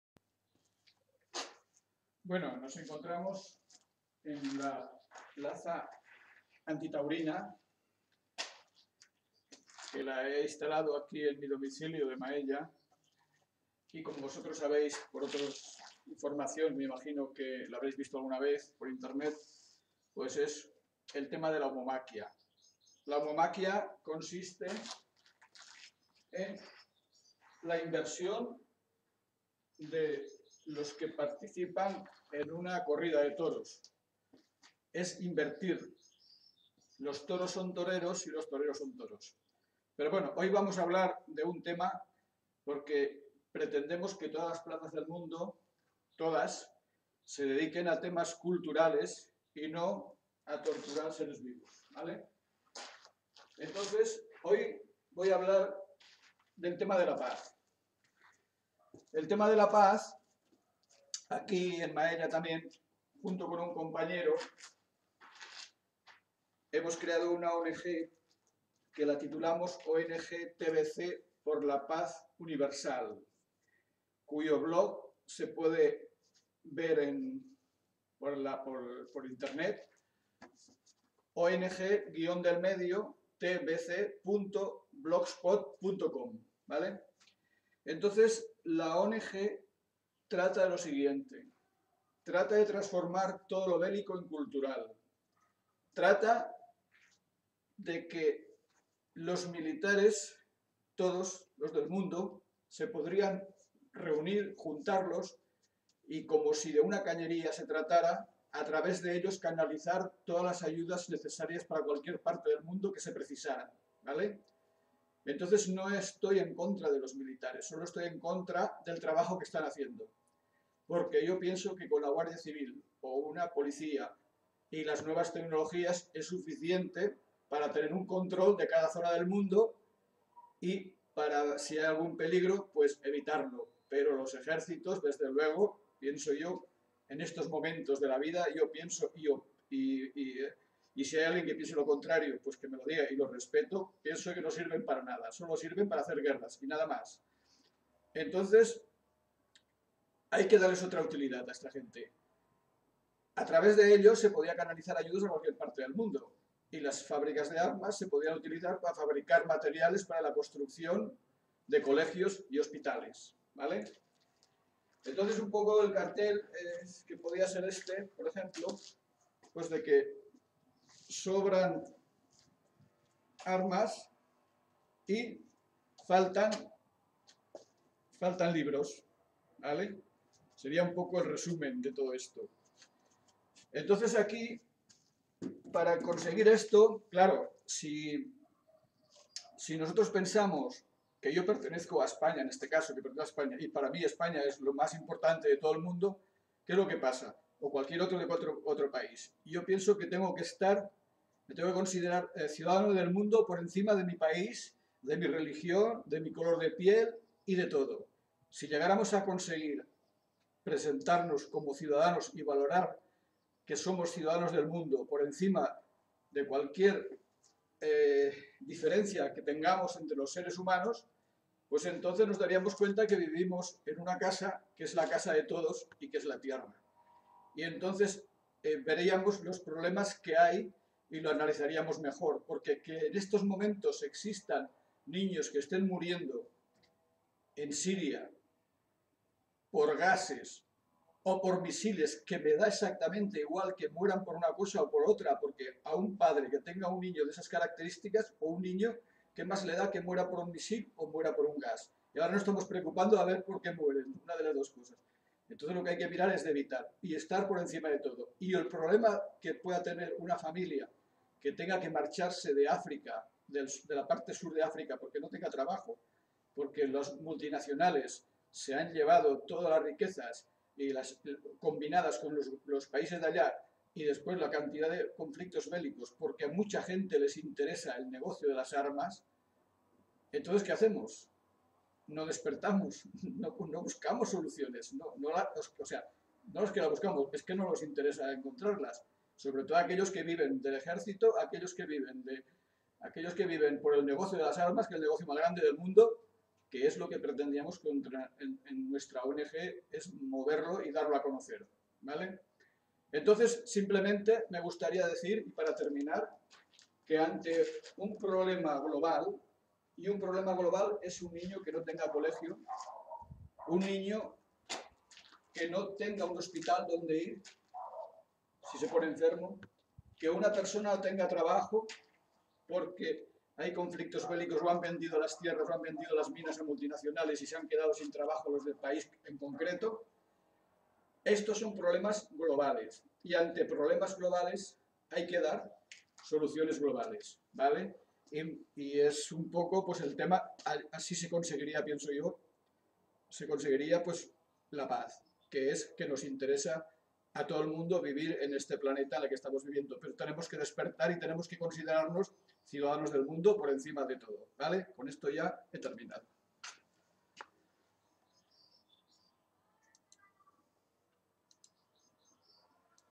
LA PAZ MUNDIAL SOLUCIÓN .
Formula para conseguir la paz MUNDIAL entre paises.
  Desde la ONG TBC POR LA PAZ UNIVERSAL quiero mostrar una formula como un tema CULTURAL desde mi espacio creativo que serviría para que aflorara la PAZ en todo el mundo .
  Uno de los pilar para que aflorara la PAZ EN TODO EL MUNDO seria la de darle otra utilidad a todos los ejercitos juntos del planeta y utilizarlos como si de una CAÑERIA se tratara y a traves de ellos canalizar todas las ayudas que se precisen en cualquier lugar del mundo .
Con todo ello conseguiríamos que todos los niños tuvieran un COLEGIO y HOSPITAL cercano ,que los nativos de cada pais no tuvieran que emigrar forzados por GUERRAS o por HAMBRE .
   En definitiva seria la mejor solución para atajar los problemas GLOBALES .en todo el mundo ya que tenemos esa herramienta de los ejércitos que si le cambiaramos su utilidad el mundo viviria en armonia y en paz.